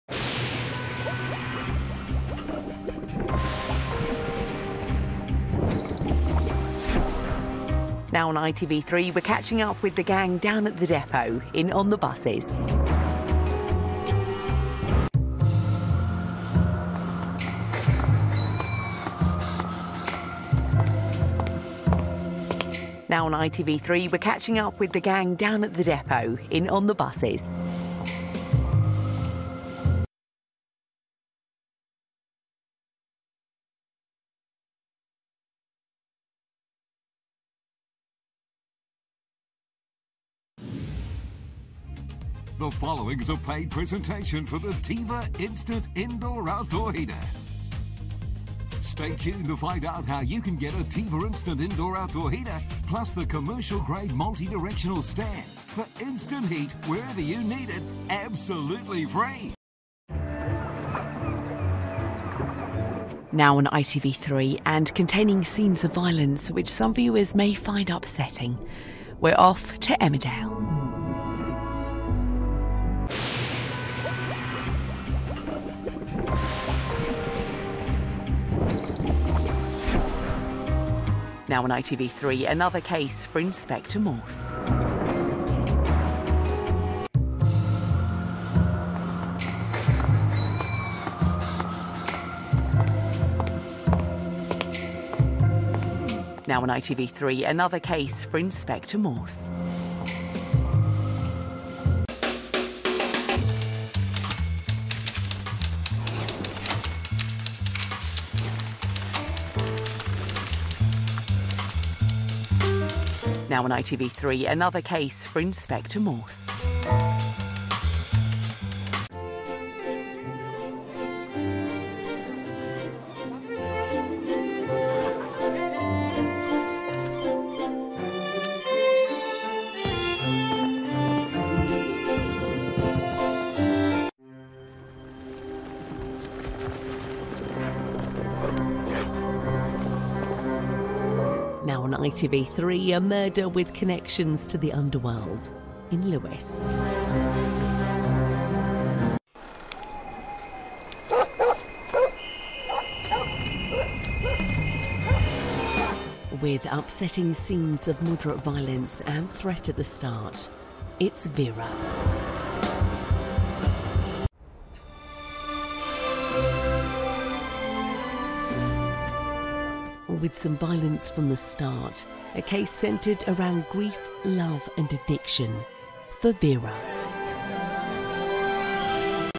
Every ITV3 ident that aired on Sunday 15th May 2022
Done as requested repeatedly by so many people. Done on this day specifically as requested by Kelly Beerling, as this day was the 6 year anniversary of the Bob's Burgers episode The Horse Rider-er.

There admittedly weren't very many idents on this day, as ITV3 kept broadcasting programmes that were 2+ hours long (and there's only so many of those that can fit into the schedule of a single day). It just is what it is I suppose.

Timestamps:
0:00 - On the Buses (1.10am)
0:15 - On the Buses (1.45am)
0:30 - Unwind With ITV (2.10am)
0:40 - Teleshopping (2.30am)
1:00 - Emmerdale Omnibus (6.00am)
1:13 - Inspector Morse (8.55am)
1:27 - Inspector Morse (11.10am)
1:42 - Inspector Morse (1.30pm)
1:59 - Inspector Morse (3.45pm)
2:14 - Lewis (6.00pm)
2:29 - Vera (8.00pm)
2:44 - Vera (10.00pm)